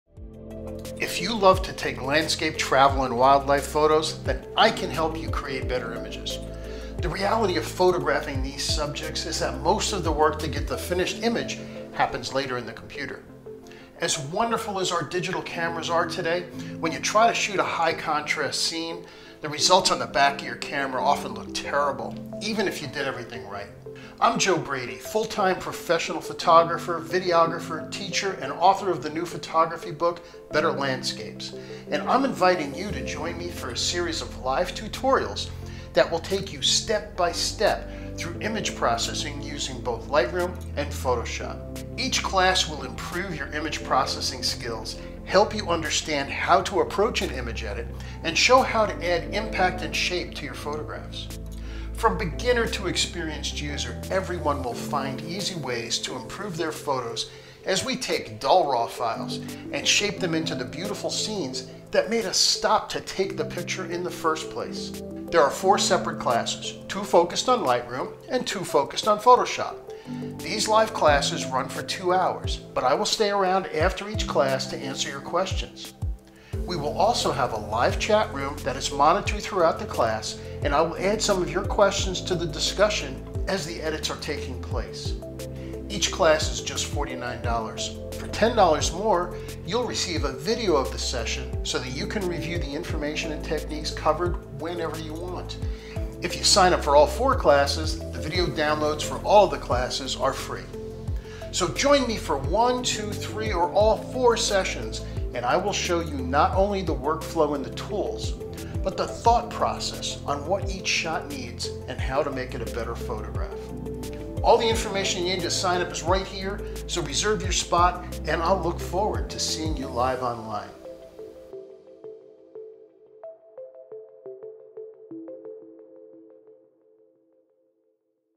Want to Master Lightroom and Photoshop?
New Online Classes to Help You Master Image Editing!

Join photographer Joe Brady as he takes you step-by-step through all the tools, techniques and tips you need to know in Lightroom and Photoshop.

In Lightroom we will start with getting organized to prevent images from getting lost, then move into the easy and common adjustments to add life to boring images. In session 2, Joe will focus on the power of the new Masking Panel and put it to use with adjustments to color and tone to make the image the best it can be.

Moving into Photoshop, you will be introduced to the 10 things that every photographer needs to know in for editing and reshaping their images. You don't need to know the 10,000 things Photoshop can do - these 10 techniques will become the best collection of tools and procedures to help you perfect your images!

There are four sessions total, and they are scheduled for a repeat the following month, so if you have to miss one in February, you can catch it in March. Sign up for all four sessions, and get the full recordings for FREE!

Registration details are on the introduction page, so sing up for your spot now!